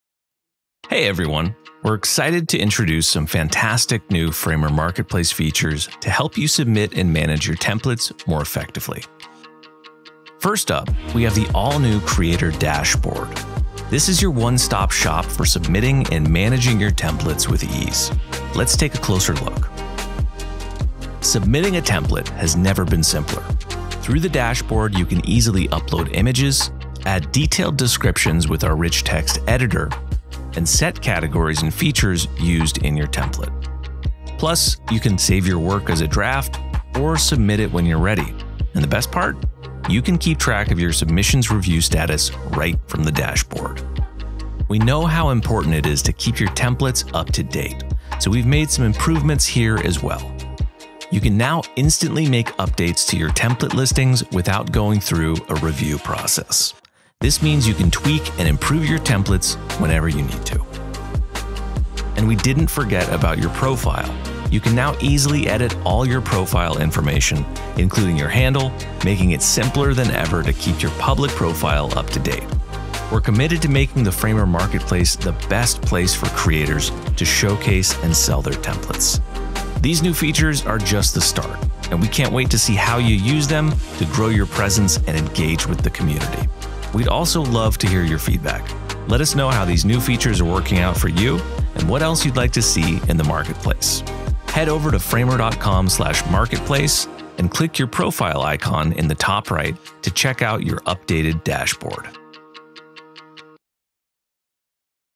Framer Marketplace Update: Creator Dashboard
Discover the exciting new features in the Framer Marketplace designed to help you submit and manage your templates more effectively! We’ve launched an all-new Creator Dashboard, your go-to spot for easily uploading images, adding detailed descriptions, and setting categories for your templates. You can now track your submission statuses and make instant updates to your listings without a review process, allowing you to keep your templates fresh and up-to-date. Plus, you can effortlessly edit your profile information to maintain an updated public profile. These enhancements are just the beginning, and we can’t wait to see how you’ll use them to expand your reach and connect with the community. Check out your updated dashboard on Framer to get started!

MB01XJOUFFGVHGT